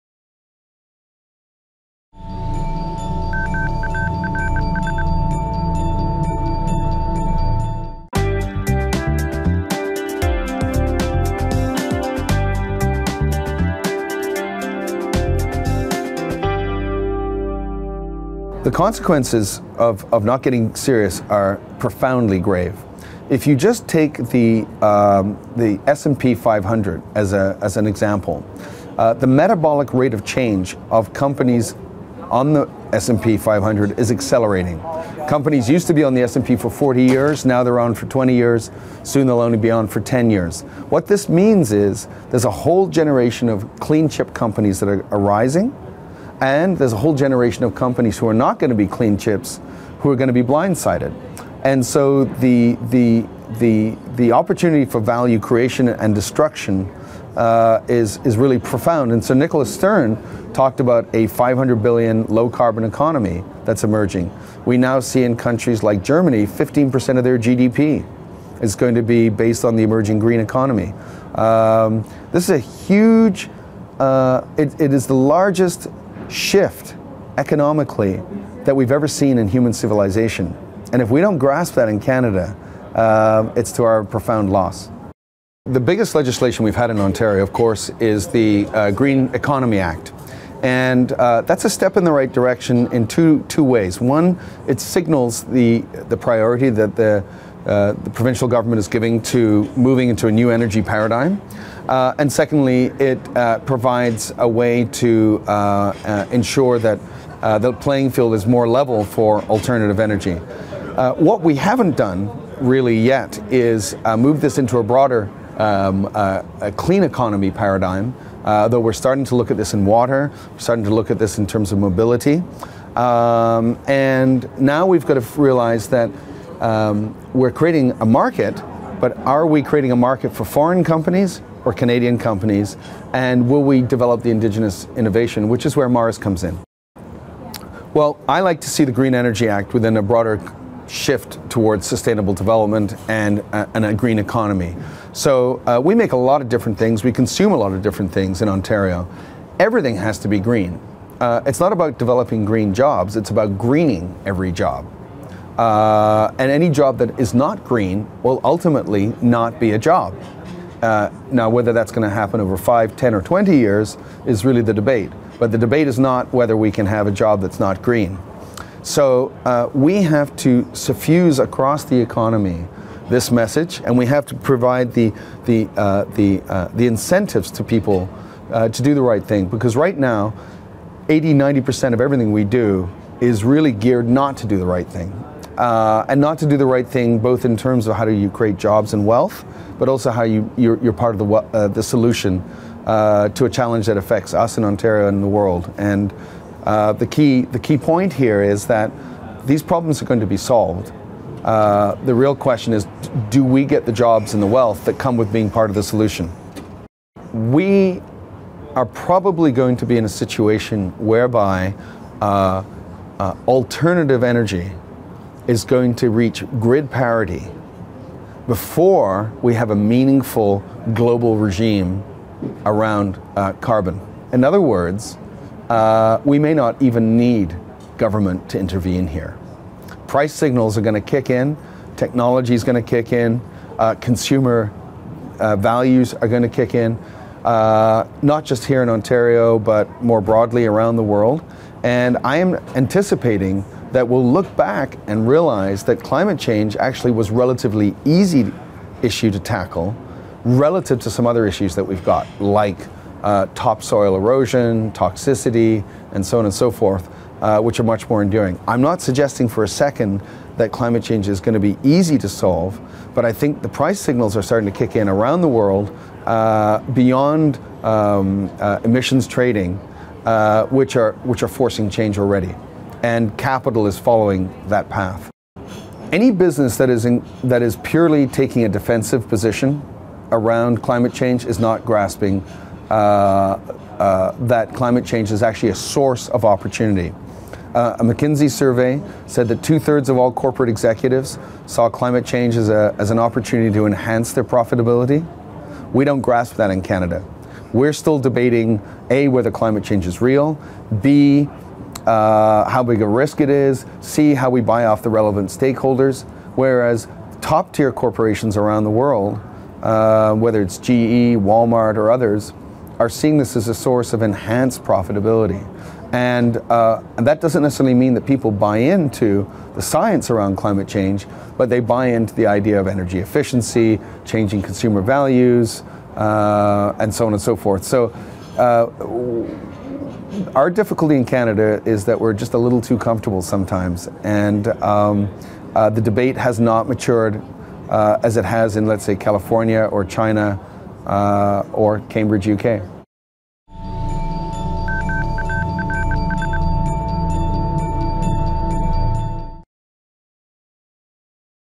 Canadian Cleantech Summit - Nicholas R. Parker
Cleantech industry observer
Canadian Cleantech Summit interview with Nicholas R. Parker, Executive Chairman, The Cleantech Group.

MaRS - Building Canadas next generation of global technology companies. marsdd.com